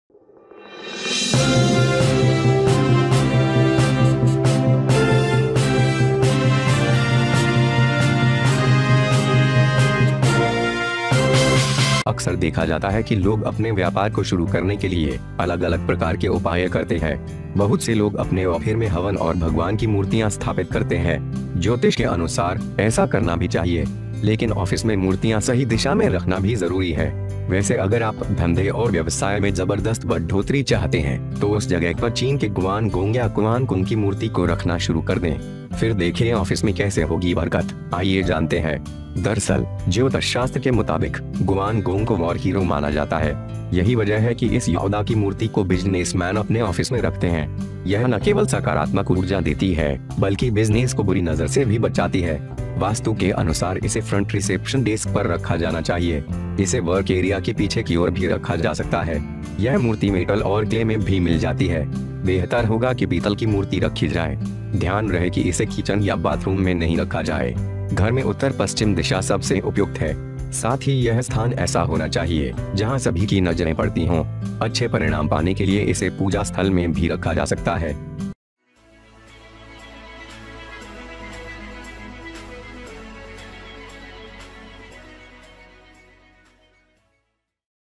ऑफिस में तरक्की के लिए रखे ये मूर्ति, बरसेगा पैसा और शौहरत
It is often seen that people take different types of measures to start their business. Many people install havans and idols of God in their offices.
According to astrology, it should also be done. But it is also important to keep idols in the right direction in the office.
Well if you want to grow tremendously in the business and business, then start keeping the statue of China's Guan Gong or Kuan Kung at that place. Then see how it will be in the office, let's know. Actually, according to Jyotosh Shastra, Guan Gong is considered a war hero. That is the reason that the businessman of this warrior kept his office in his office.
It not only gives positive energy but also saves the business from a bad point of view. According to Vastu, it should be kept at the front reception desk.
It can also be placed on the back side of the work area. This statue is also found in metal and clay. It would be better if a bronze statue be kept. Keep in mind that it is not kept in a kitchen or bathroom. The North-West direction is most suitable in the house. At the same time, this place should be where everyone has eyes. To get good results, it can also be placed in the place of worship.

HJ NEWS !!